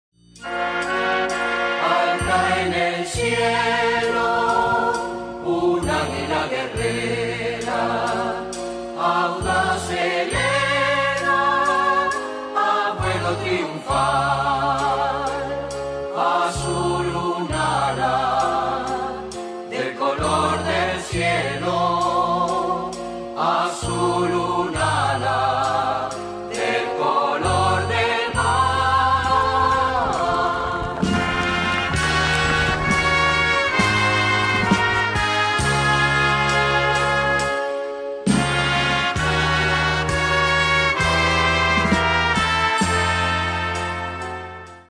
Capital celebró 250 años del nacimiento de Belgrano
En la plaza que lleva el nombre del prócer, el intendente Emilio Baistrocchi encabezó el reconocimiento al creador de la bandera argentina.